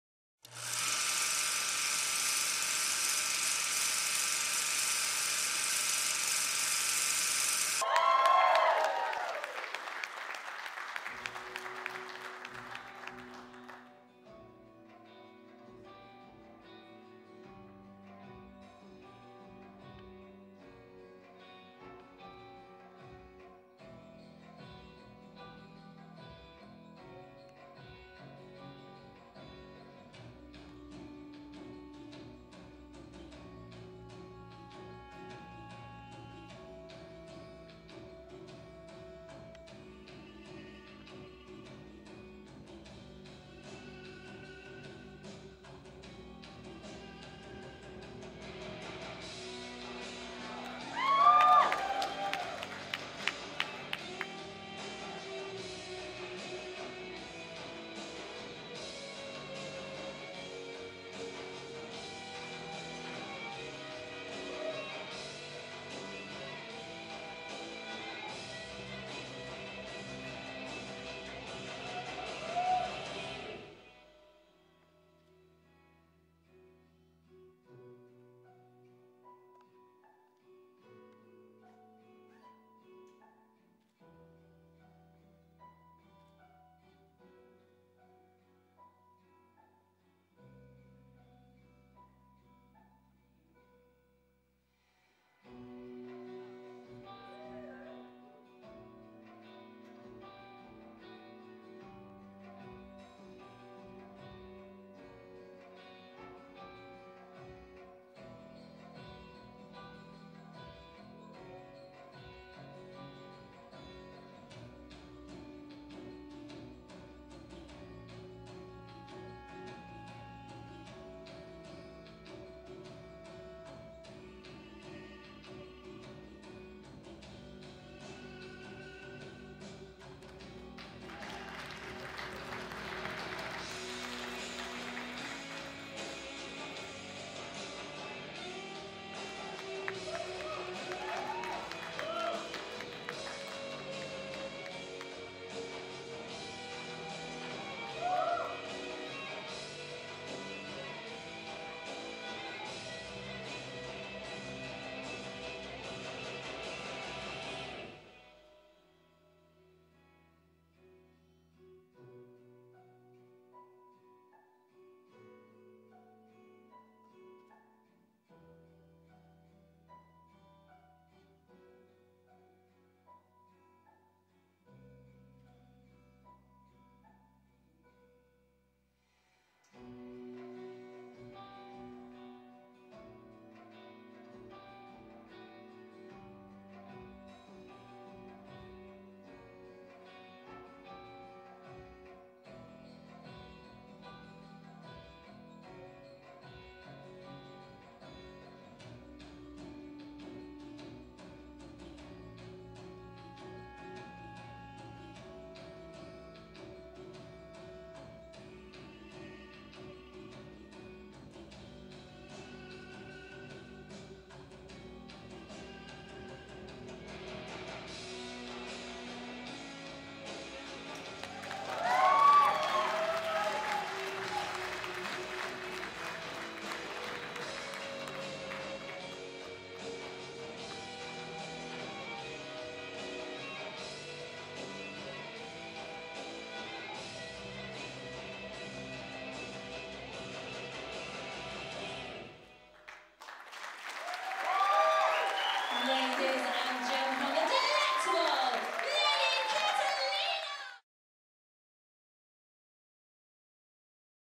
Lady Catalina Aerial Hoop performance from the Cream Tease
The Cream Tease Burlesque Matriarch, Lady Catalina performs her gravity defying Aerial hoop routine for your enjoyment. Filmed and processed by Stewart Gibbs Photography.